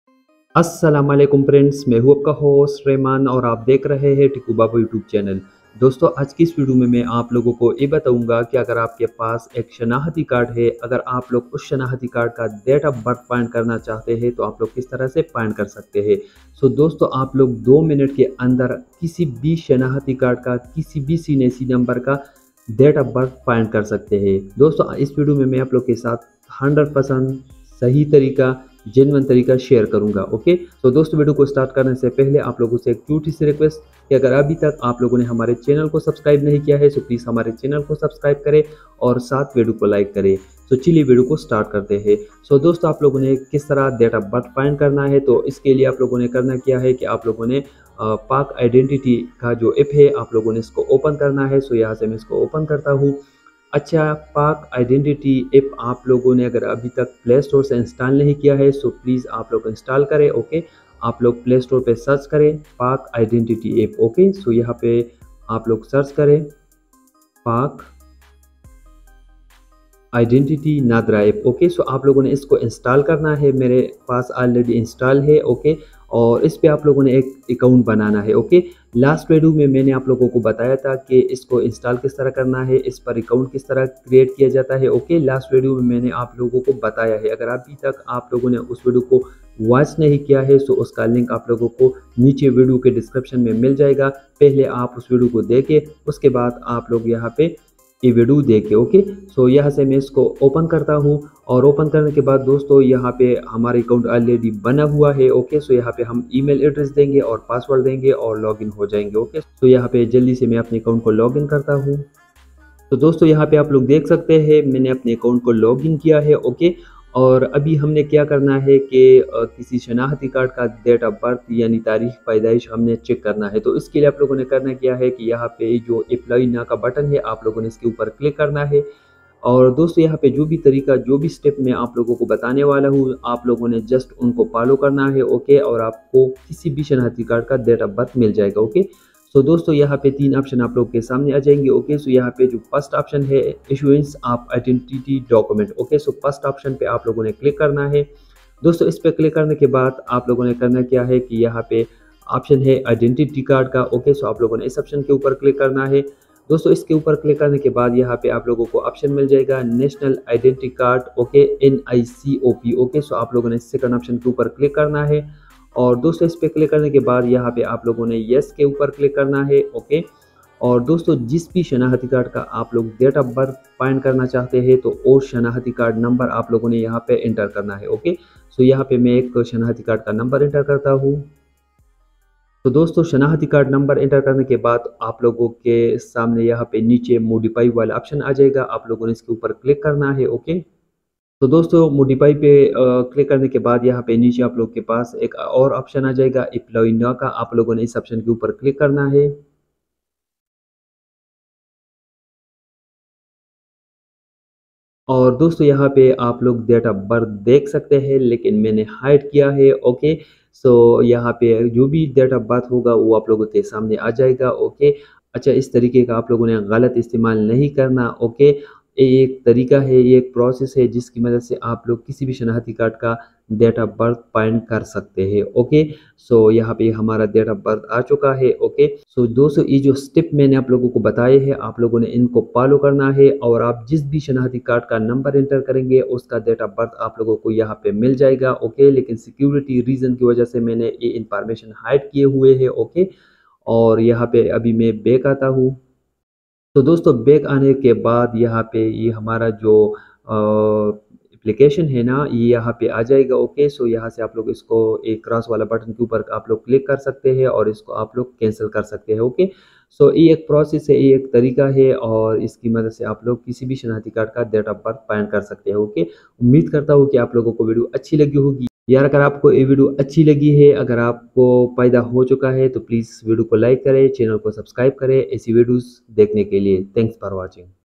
How to Get Date of Birth of Cnic | Date of birth cnic ka kaise find kare
So today in this video i will show you that how you can get your date of birth of your id card.so if you have cnic number and if you want to find your date of birth then this video is specially for you.

Tutorials